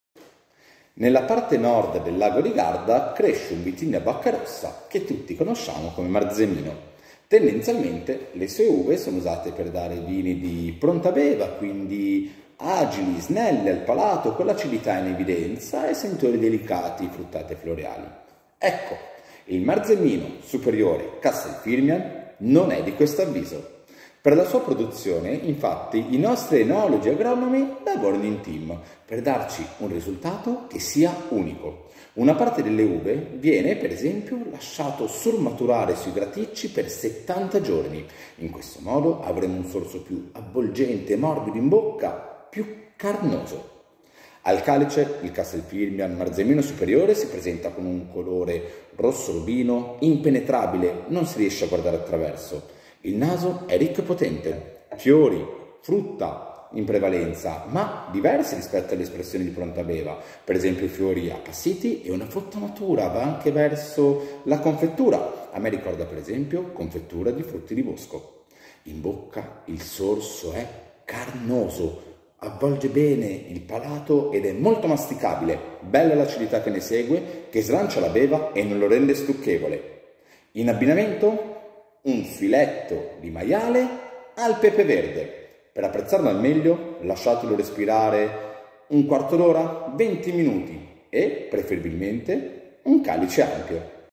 Castel Firmian Marzemino Superiore - Mezzacorona
Nuova idea 💡
90° sorso
Come nel calcio, solo i momenti salienti
Proviamo la descrizione del vino in 90 secondi...